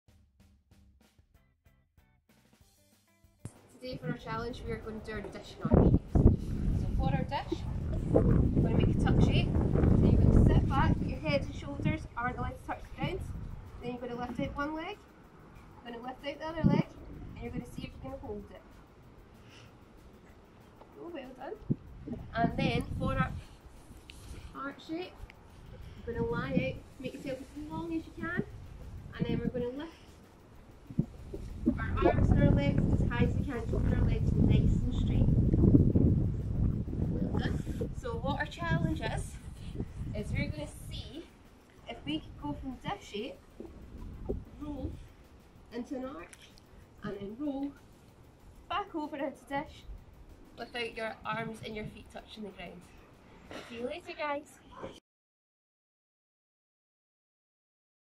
Day 6 - dish & arch
Take part in our daily gymnastics challenge!
Today is dish and arch.

Let us know how you get on using